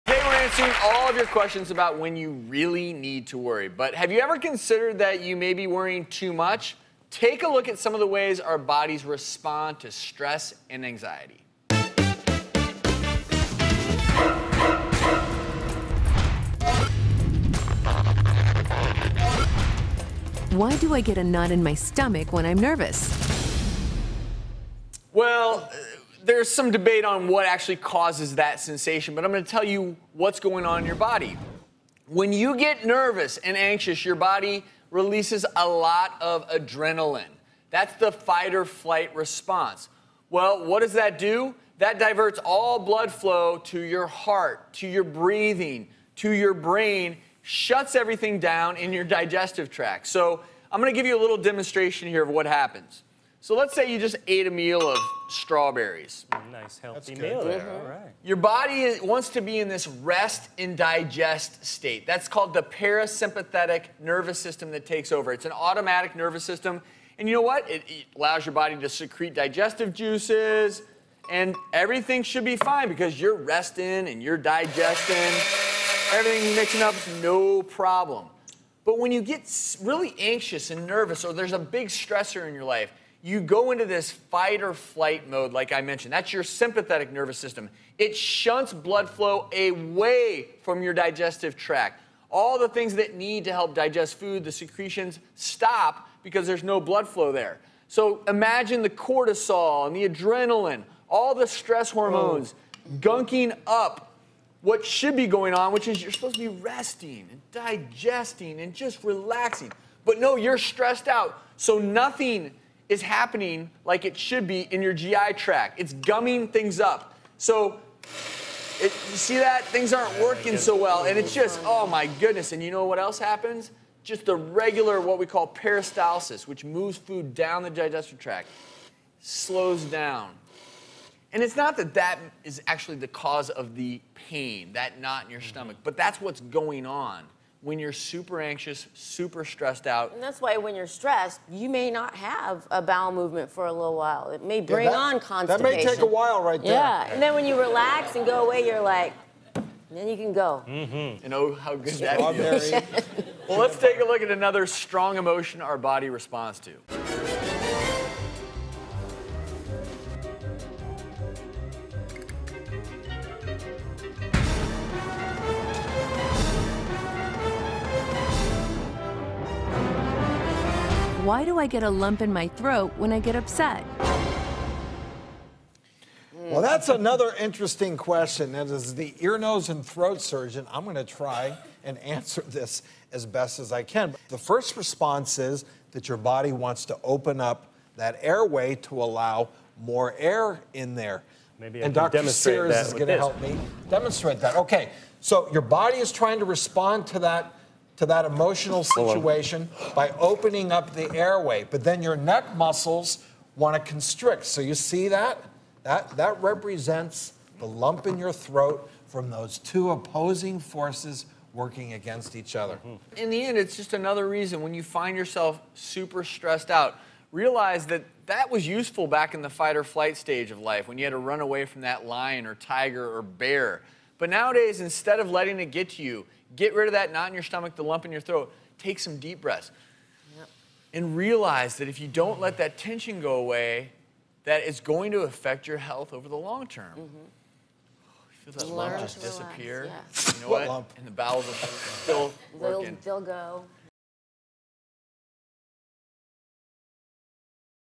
Too Much Worrying Medical Course
For Educational Use Only - Fair Use - Do you worry too much Learn what happens to your body when you get that knot in your stomach or lump in your throat and find out how to avoid them. rn